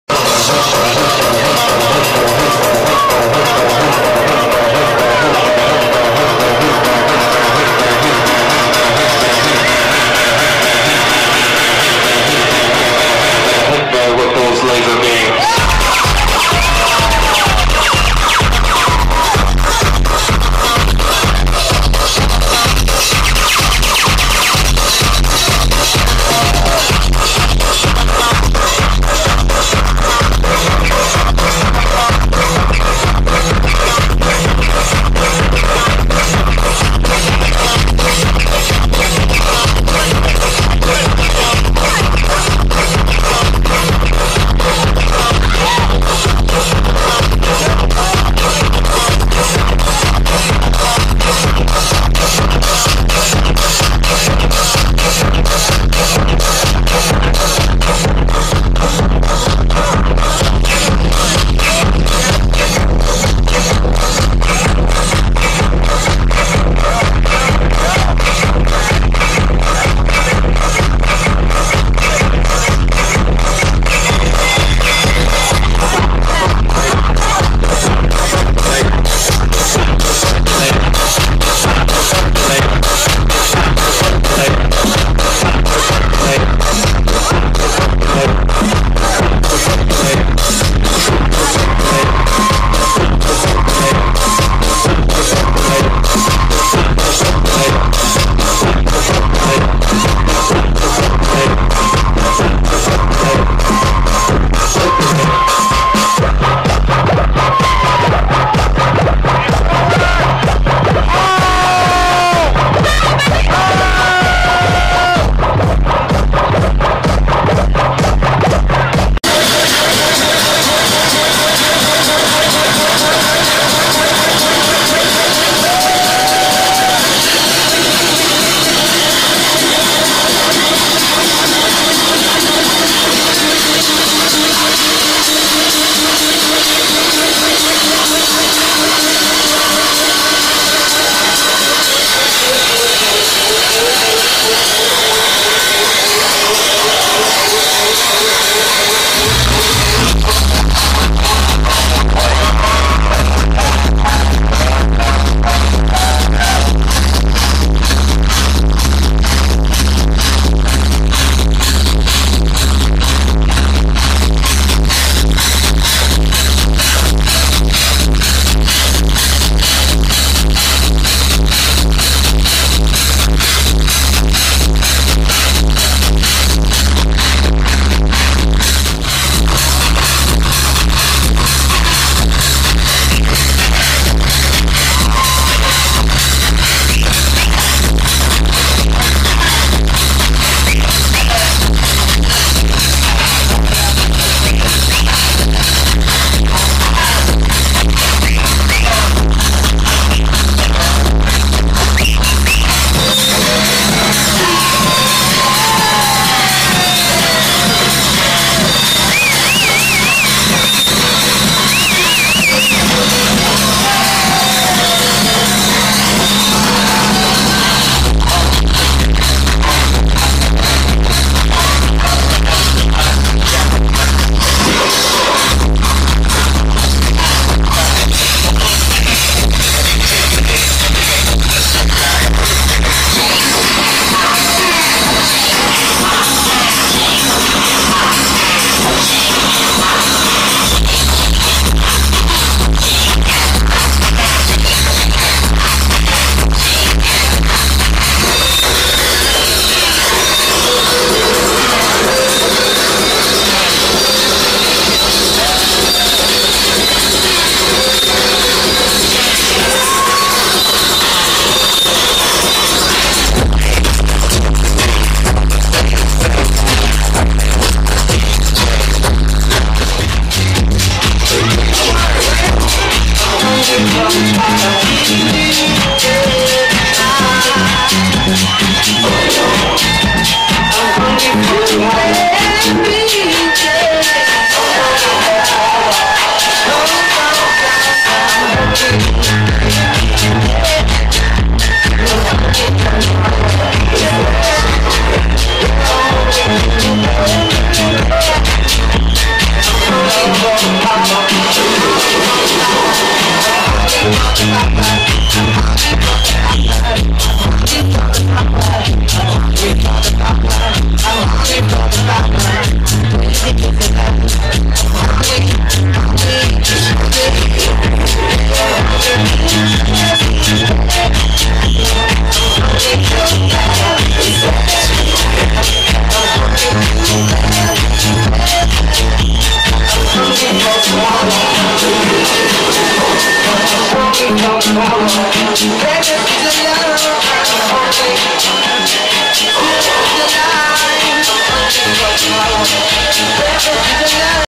CAZHMERE - CENTRE ISLAND TORONTO
ELECTRIC ISLAND CIVIC HOLIDAY MONDAY with GREEN VELVET, HOT SINCE 82, SOUL CLAP @ Centre Island